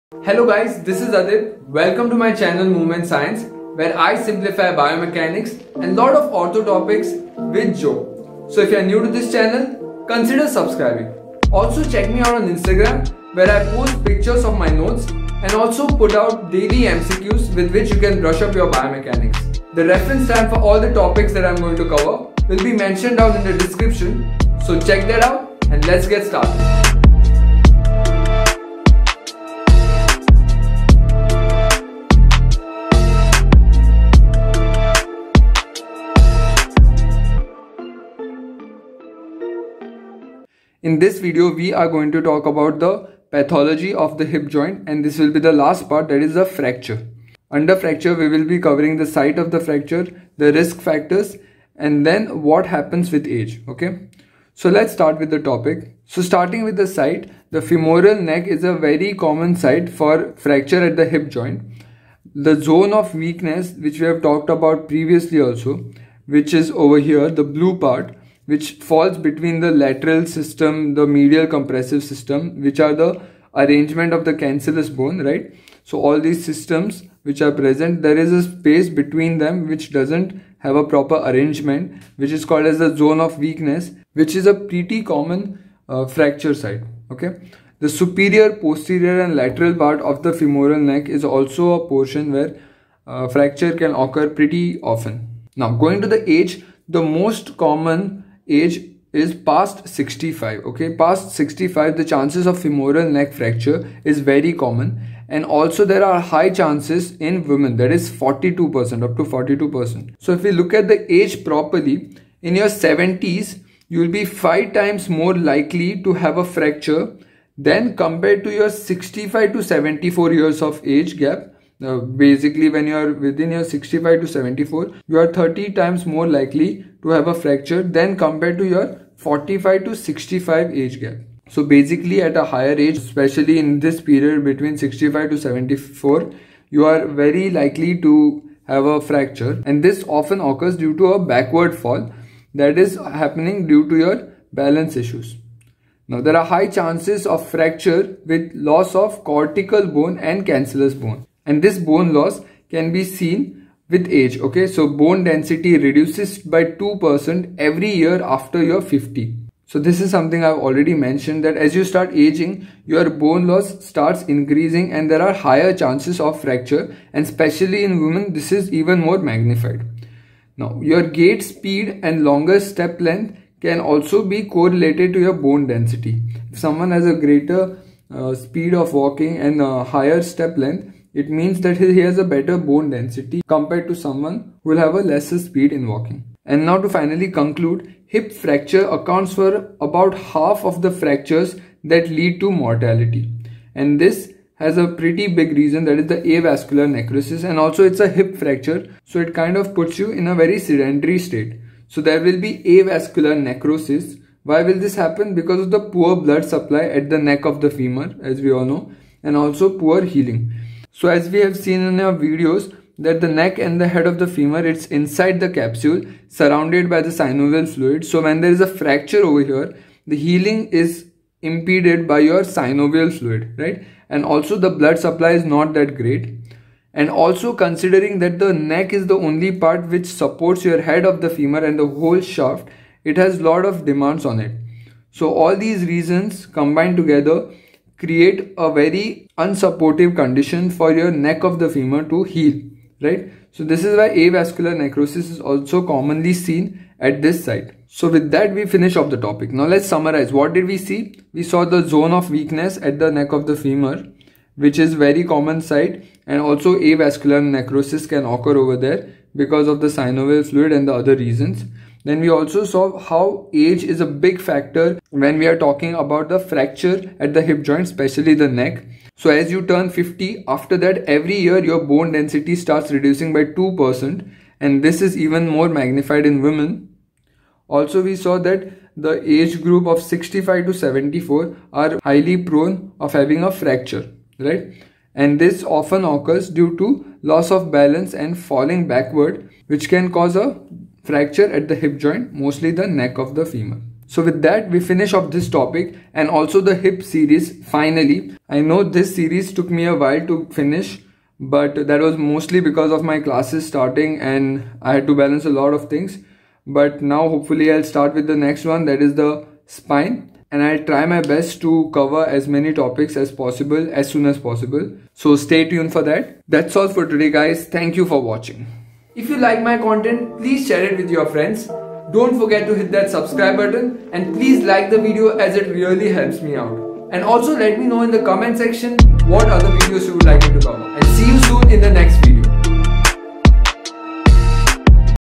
HIP PATHOLOGY FRACTURE (HIP COMPLEX BIOMECHANICS)Physiotherapy Tutorial
summary of the video with the time for reference:

1.Site of the Fracture 0:55
-femoral Neck
-Zone of weakness
-Superior, posterior and lateral pat of femoral Neck

2.Risk factors 1:35
-Higher in women
-High risk between age group of 65 and 74
-Bone density reduces by 2 % every year after 50
-Mostly occurs with backward fall

3.High Mortality 3:06
-Avascular necrosis because of compromised blood supply
-improper healing due to synovial fluid